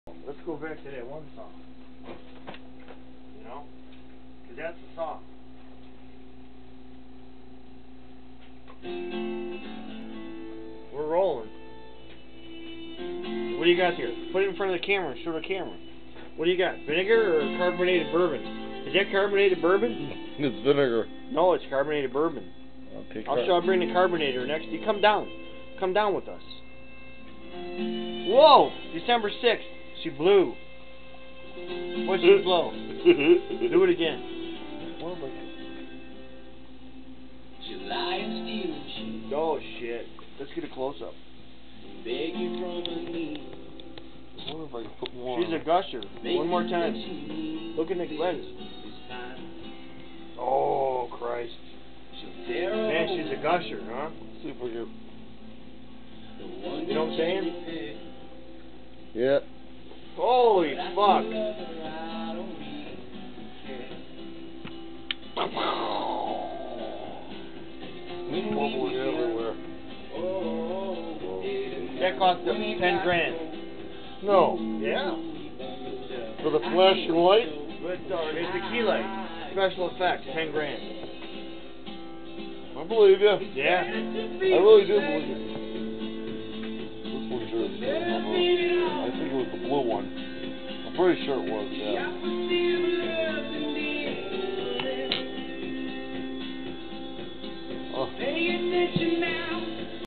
Freddie's Fury: Volcano Vodoo Server Spill December 6, 2012 per Revelations 6
Watch as the Evil Genius Freddie Borgstrom uses his charm and enchantments to erupt the active Volcano in Yosemite Park on his Uncle Dicks' Birthday December 6th, 2012 to coincide with Revelations 6 where the skies darken and the moon turns to blood.